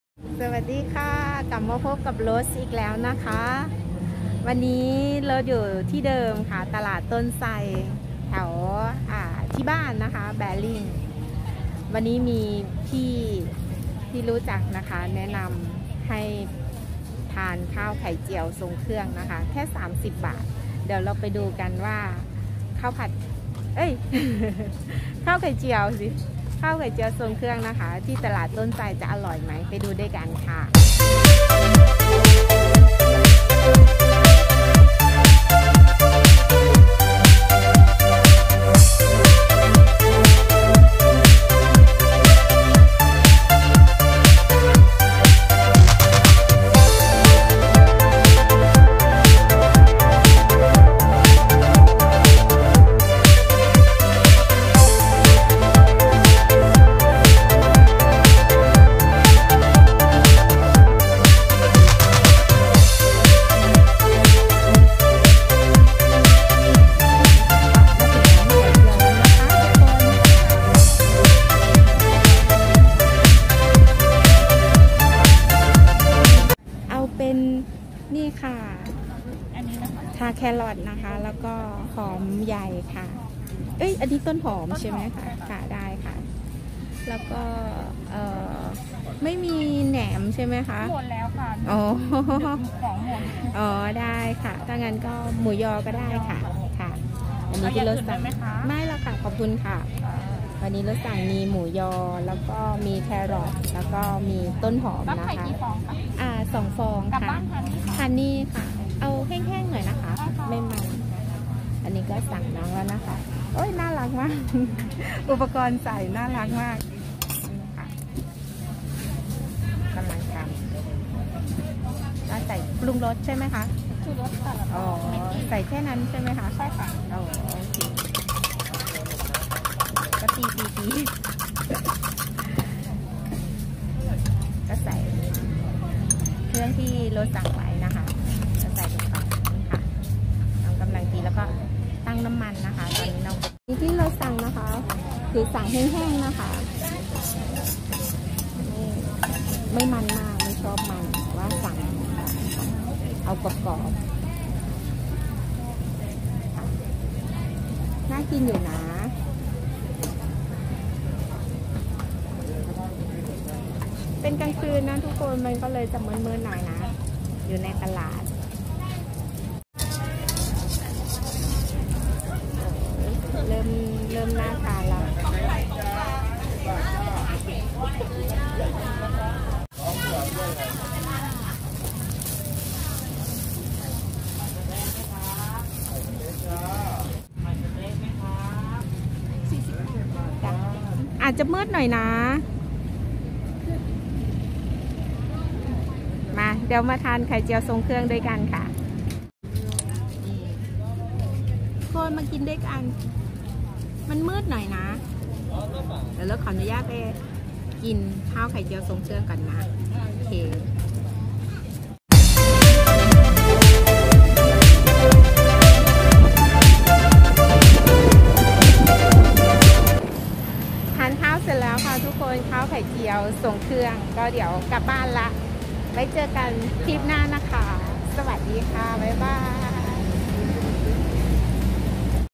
ข้าวไข่เจียวทรงเครื่องตลาดต้นไทร แบริ่ง
วันนี้พาทุกคนมาเดินเล่นตลาดต้นไทรอีกแล้วค่ะแต่ไปกว่านั้นเดี๋ยวจะพาทุกคนไปทานข้าวไข่เจียวทรงเครื่องค่ะถือว่าเจ้านี้เด็ดมากค่ะในตลาดต้นไทรแบริ่งมีน้ำพริกกะปิให้ด้วยนะคะทุกคนเดี๋ยวเราไปทานข้าวไข่เจียวทรงเครื่องด้วยการค่ะ ถึงแม้ว่าวันนี้ที่ไปน้ำพริกกะปิจะหมดแล้วนะคะแต่ว่าโรสก็ได้ทานข้าวอร่อยๆนะคะเป็นข้าวไข่เจียวทรงเครื่องข้าวหอมมะลิราคาเริ่มต้นแค่ 30 บาทถ้าขาย 2 ฟองก็ 40 บาทค่ะ streetfood ตลาดสด ถนนคนเดิน ข้าวไข่เจียวทรงเครื่อง @ROSEONTRAINนั่งรถไฟไปเรื่อย